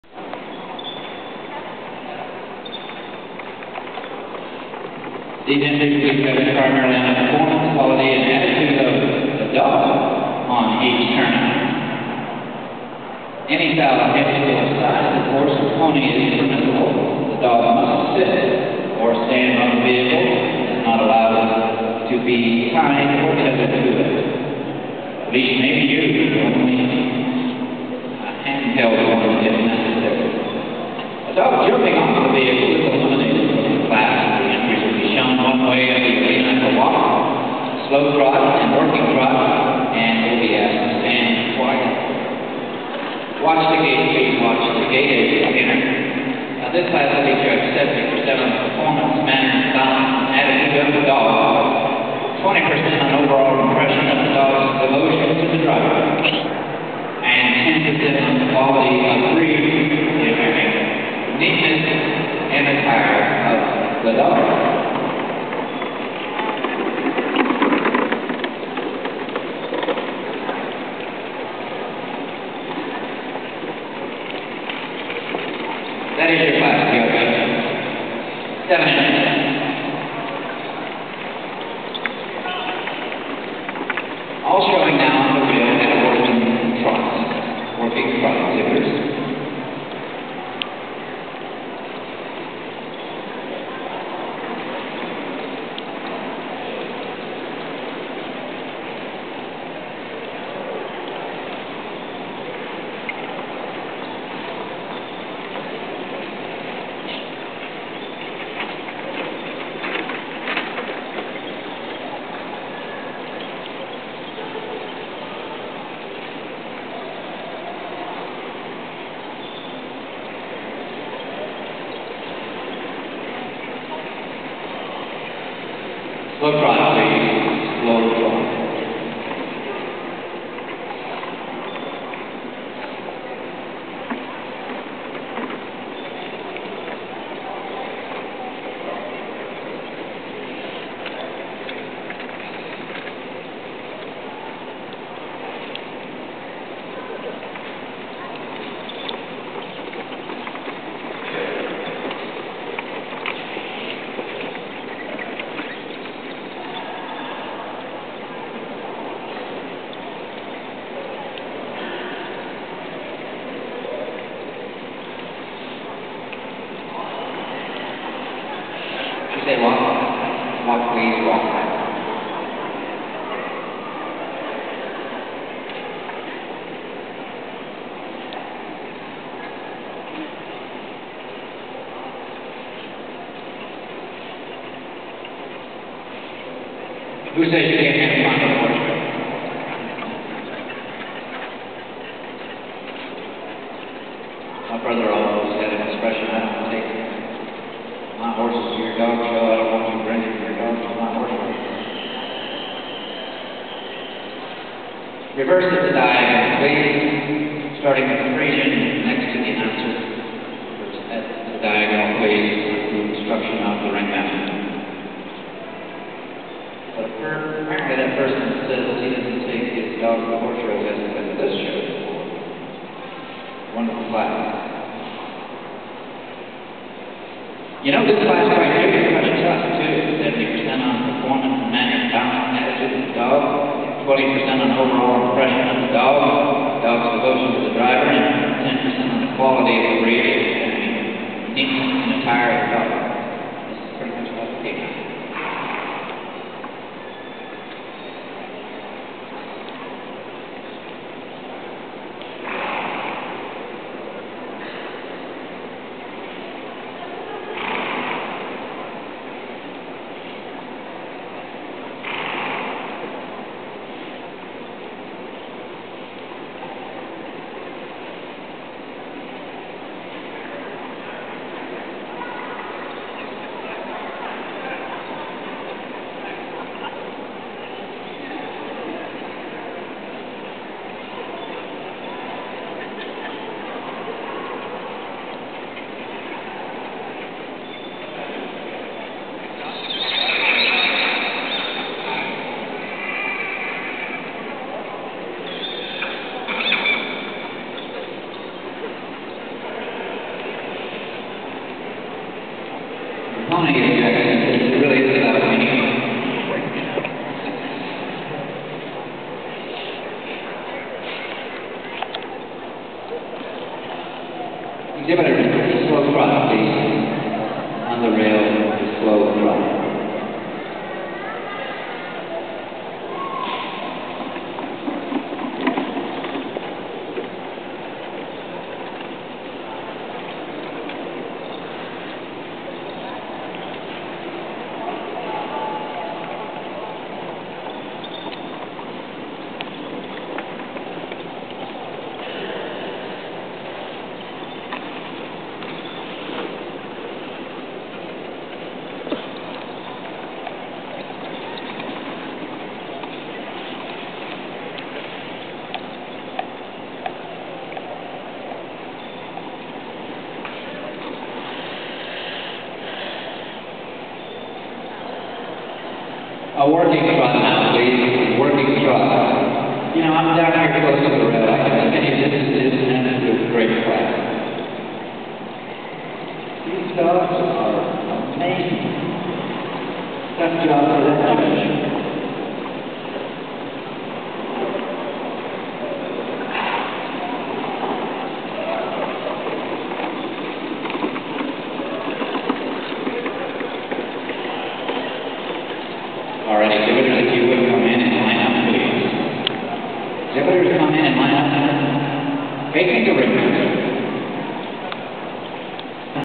CAA Carriage Classic 2011 - carriage dog class
The Beagle won the class!  The big dog on the marathon vehicle came in second.

Class 26 -- Carriage Dogs Performance
1st place: Michelle Blackler and Lucy Fur
2nd place: Nancy Dozier
3rd place: Marilyn Macfarlane
4th place: Mary Thomas
5th place: Katharine Magruder
6th place: Olivia Smock